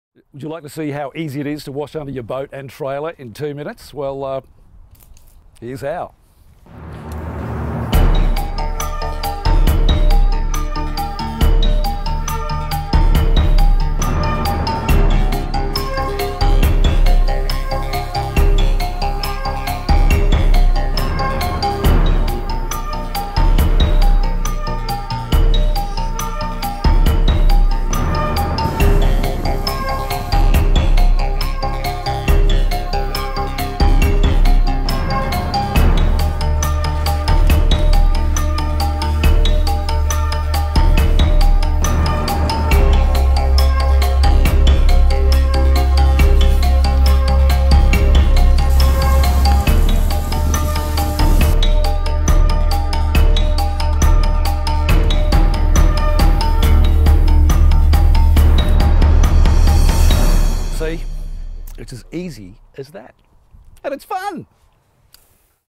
The Underwash: Washing Your Car
How to wash the underneath of your car in just two minutes... Watch on.